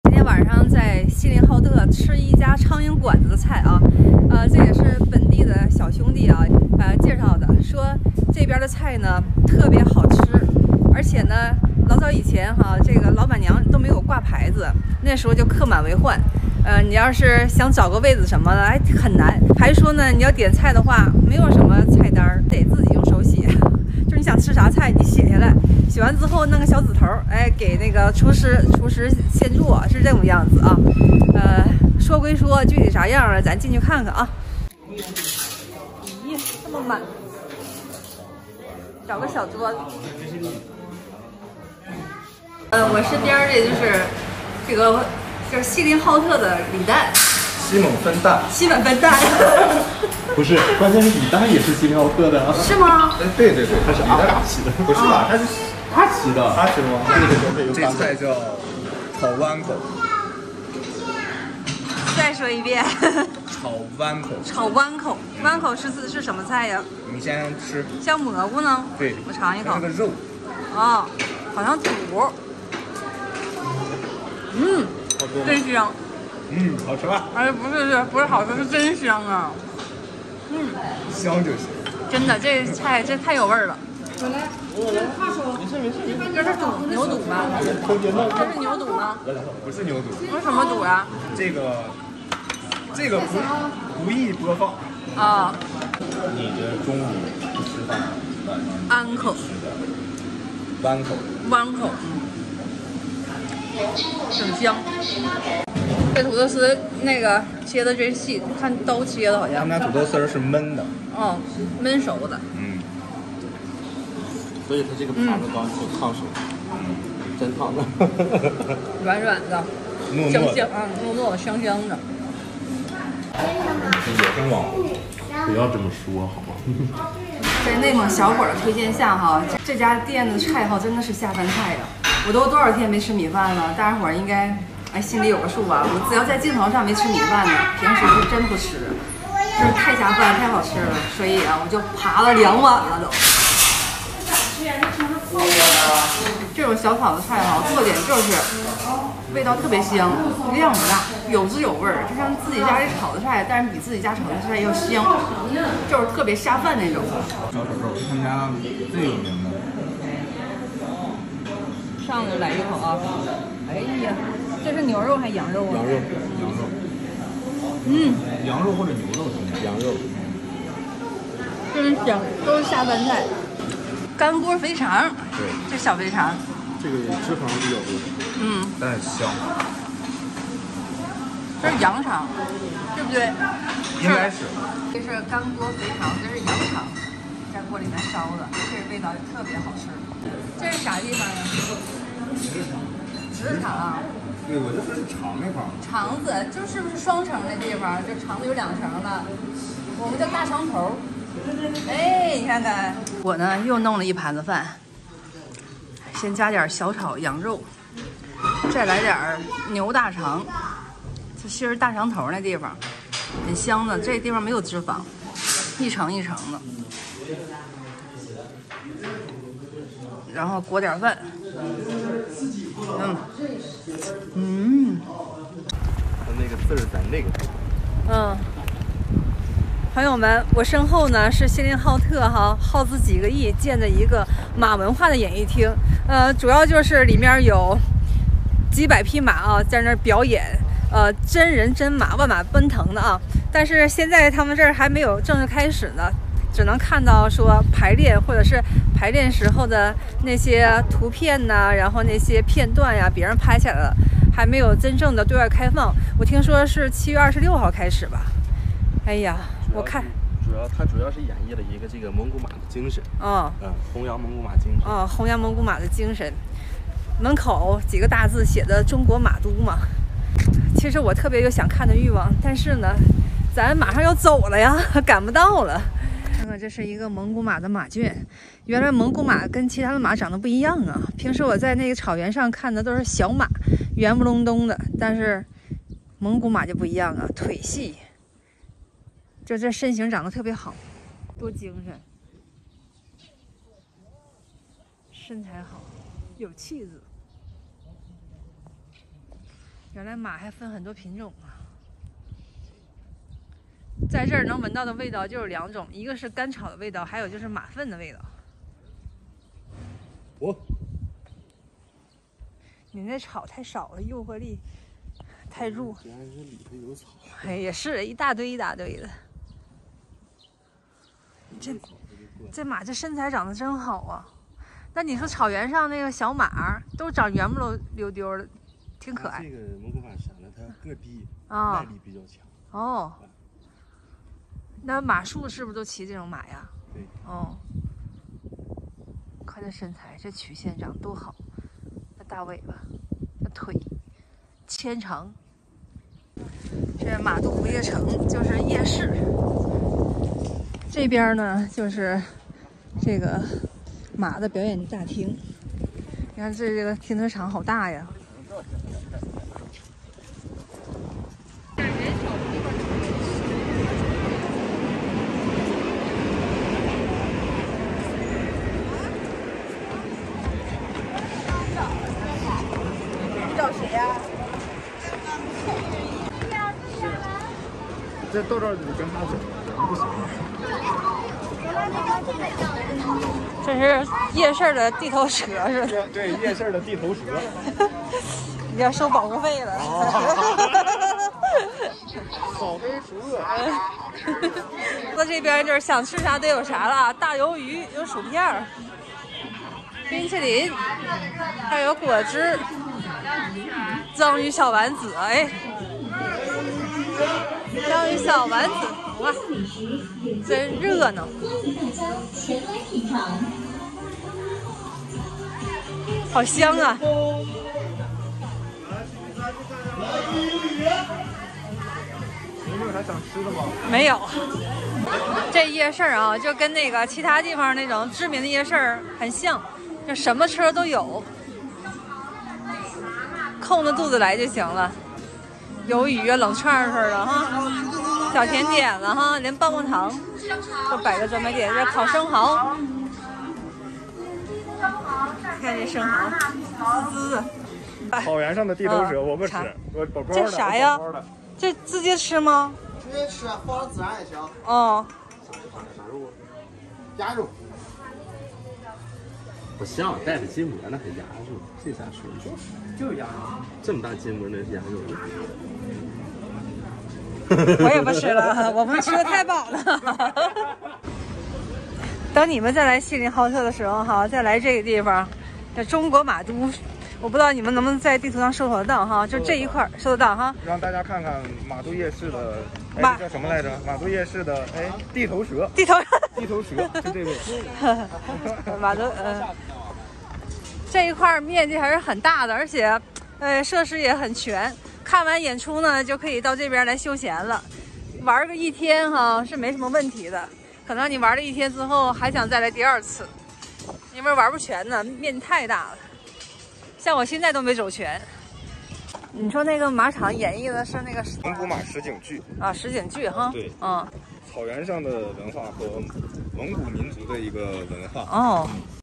和李诞去苍蝇馆子吃牛弯口，弯口到底是啥？大家都解释不清楚【莹莹】
莹莹每天与大家分享东北生活日常见闻和美食搜罗视频